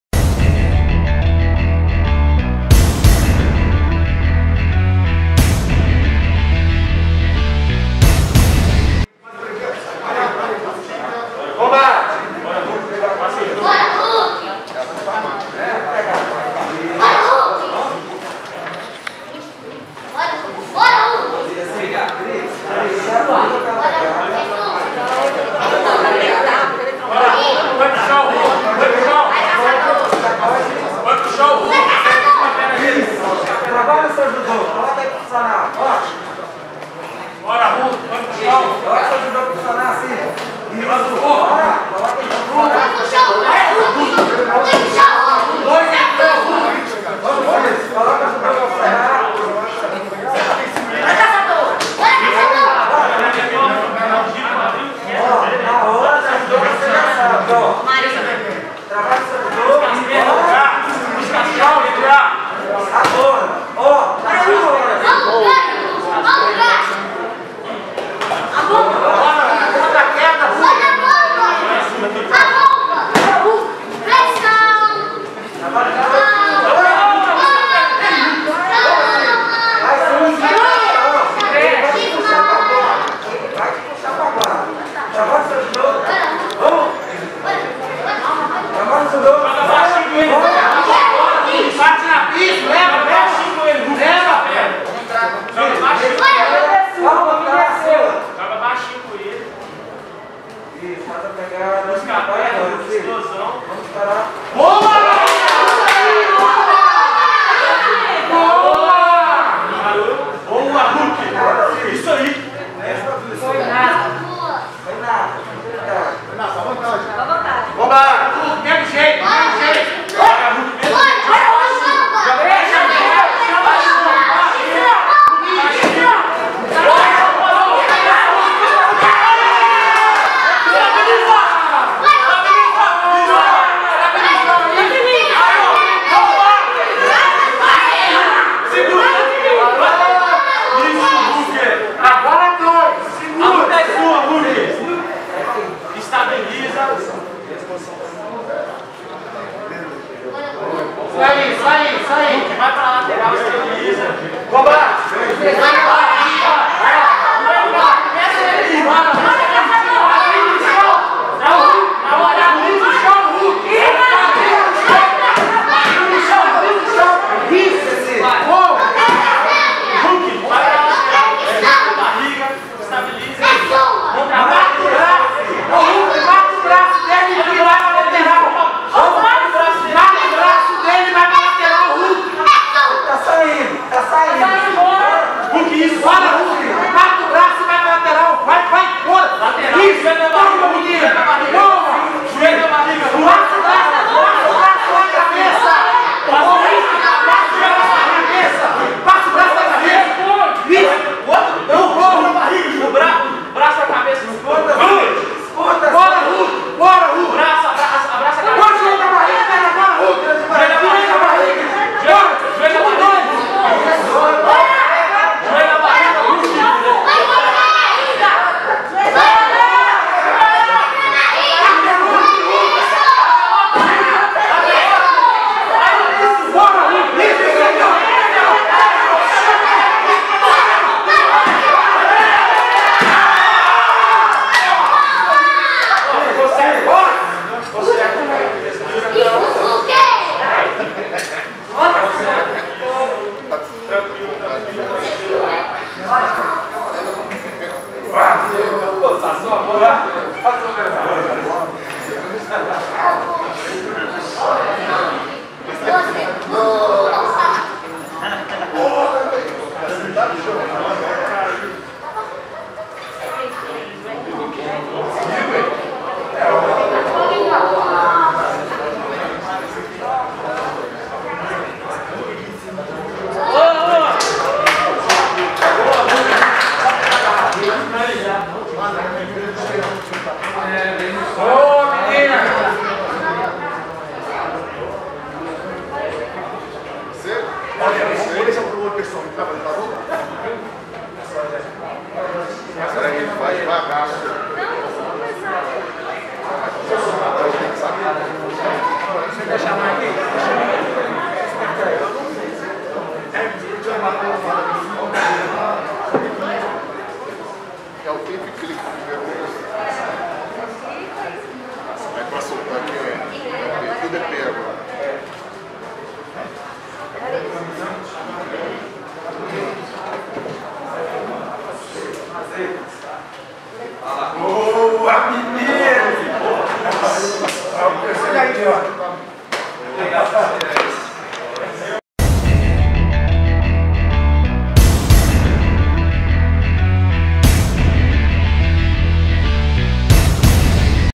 Robson Hulk vs Sidney Patrick - Copa Búfalo de Jiu-Jitsu
Robson Rodrigues Hulk (Búfalo Team) venceu Sidney Patrick (Vitor Team Jiu-Jitsu) por finalização.